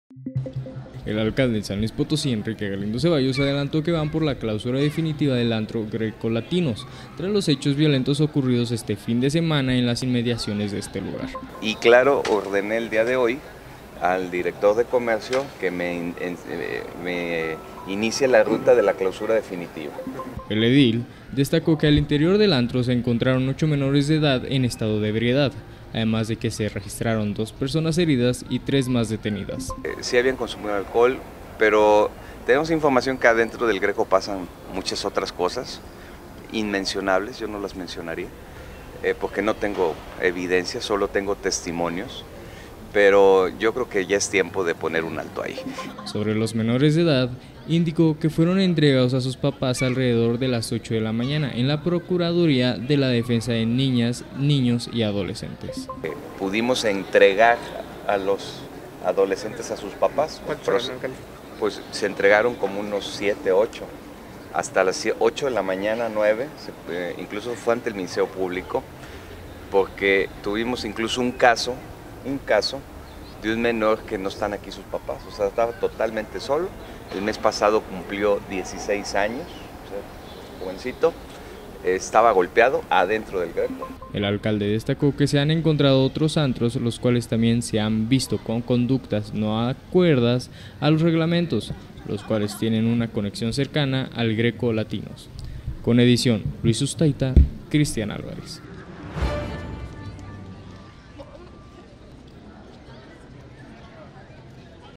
Van por clausura definitiva del Greko Latinos: Enrique Galindo
El alcalde de San Luis Potosí, Enrique Galindo Ceballos adelantó que van por la clausura definitiva del antro Greko Latinos, tras los hechos violentos ocurridos este fin de semana en las inmediaciones de este lugar.